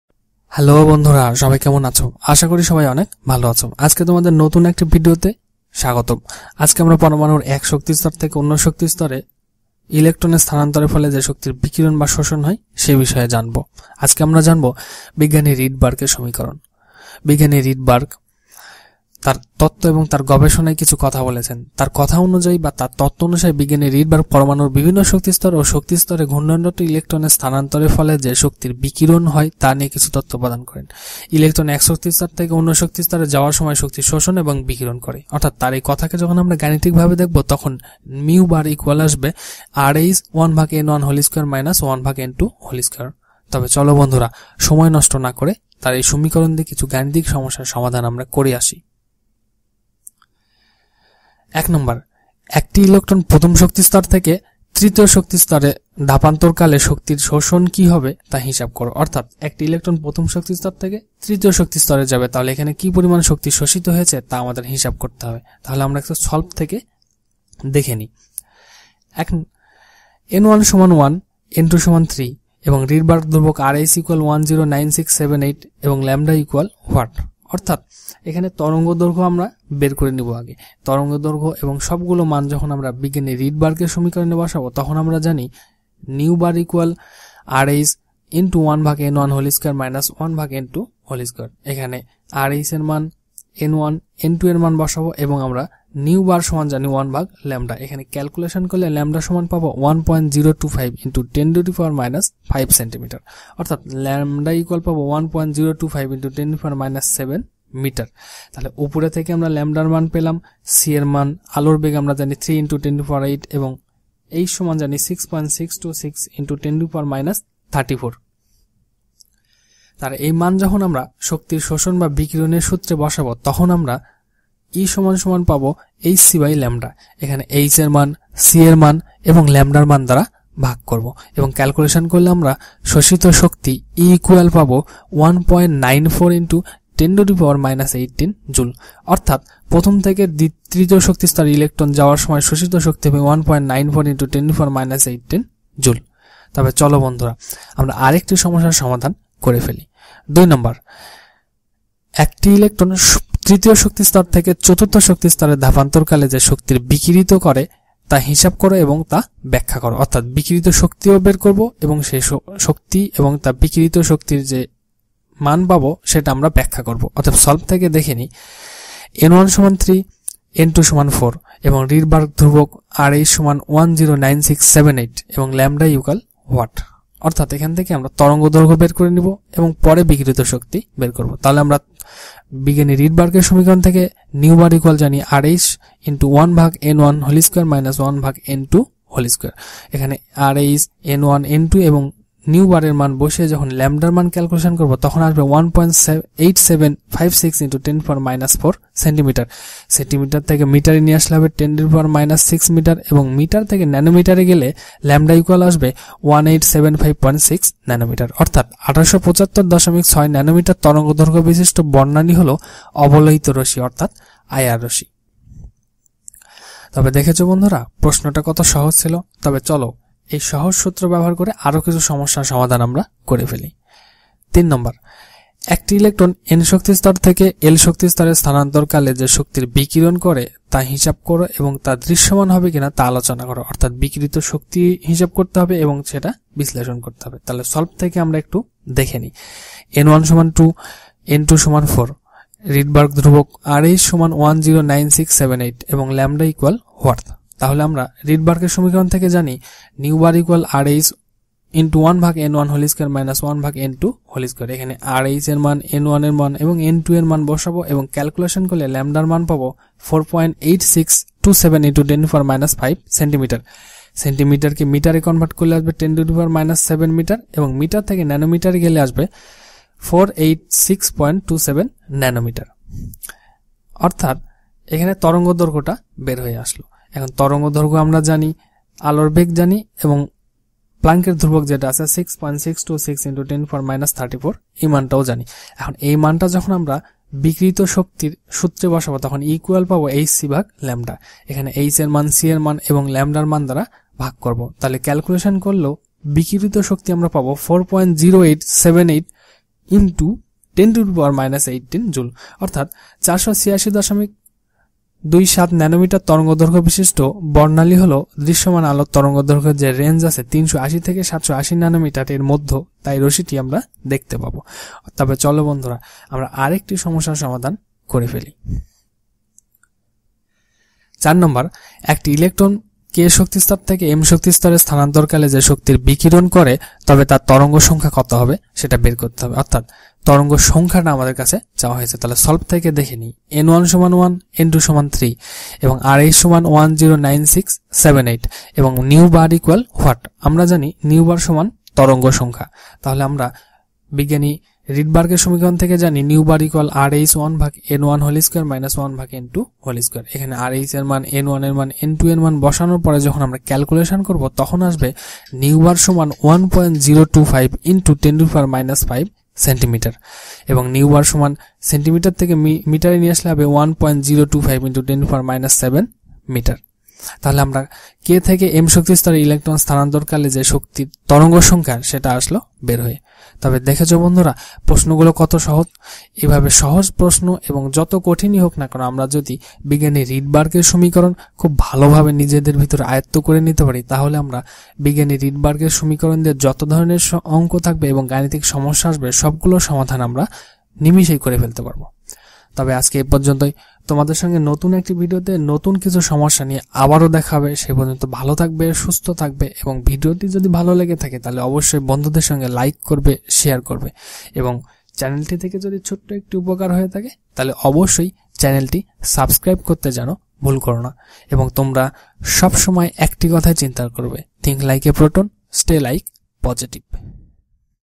Rydberg formula-Math problems solve-(রিডবার্গ এর সমীকরণ ও এর গানিতিক সমস্যার সমাধান)
In atomic physics, the Rydberg formula calculates the wavelengths of a spectral line in many chemical elements. The formula was primarily presented as a generalization of the Balmer series for all atomic electron transitions of hydrogen. It was first empirically stated in 1888 by the Swedish physicist Johannes Rydberg,[1] then theoretically by Niels Bohr in 1913, who used a primitive form of quantum mechanics. The formula directly generalizes the equations used to calculate the wavelengths of the hydrogen spectral series.